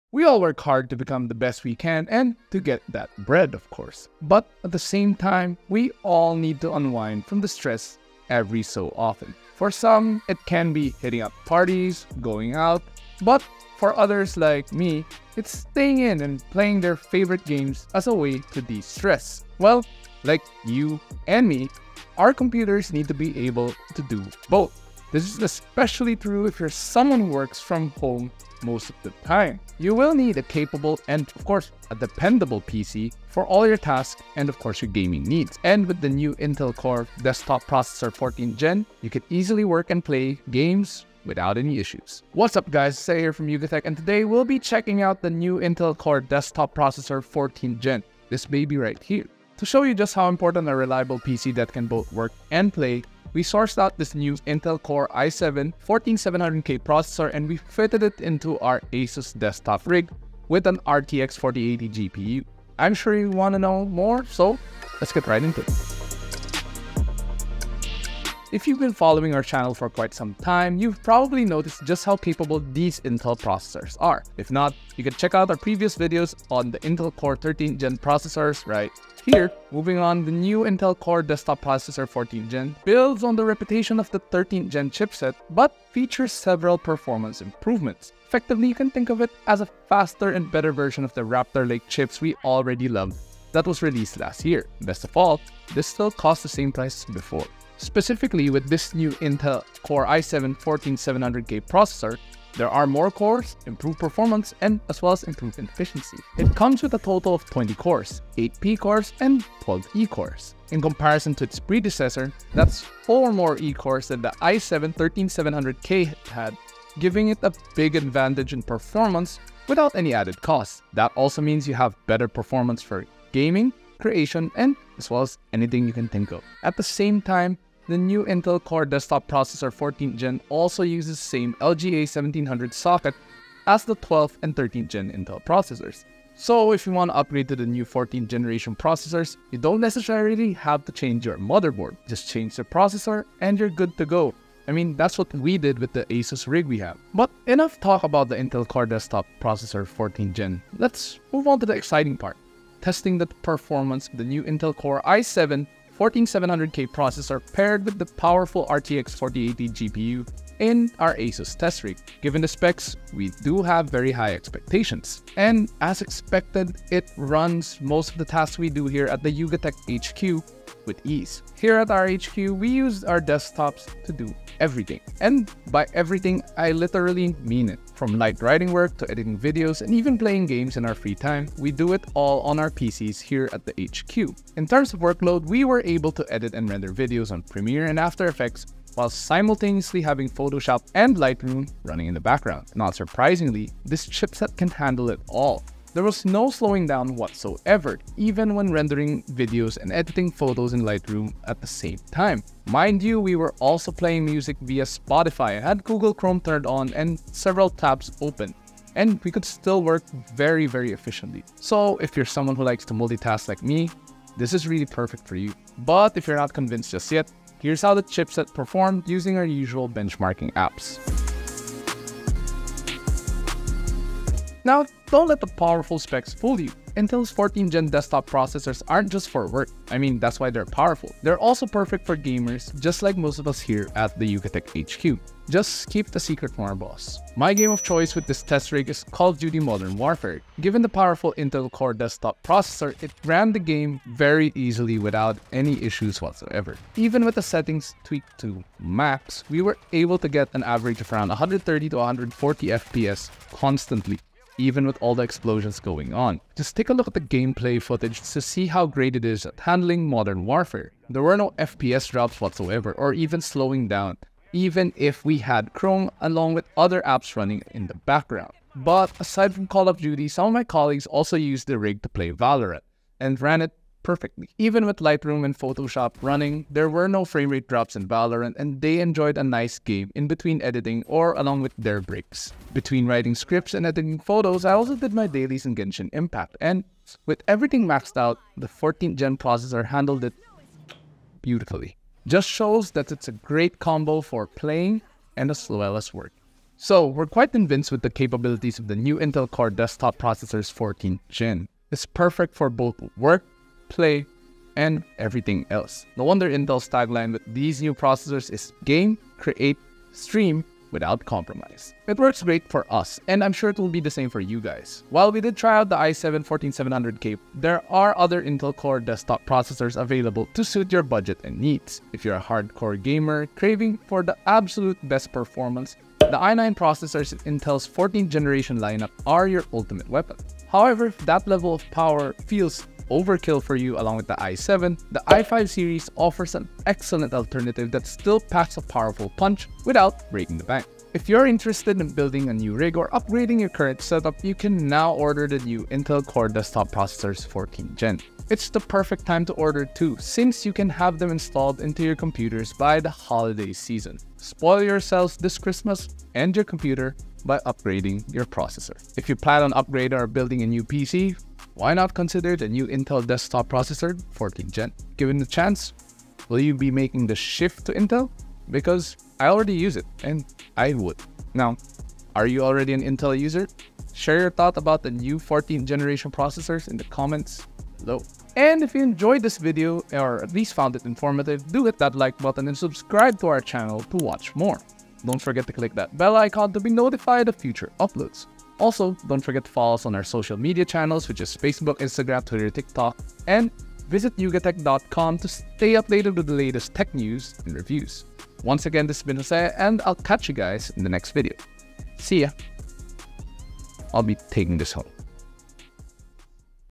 Work Hard, Play HARD with the NEW Intel Core Desktop Processors (14th Gen)
This video features Intel's latest 14th Generation Core Desktop processors, watch on to learn more!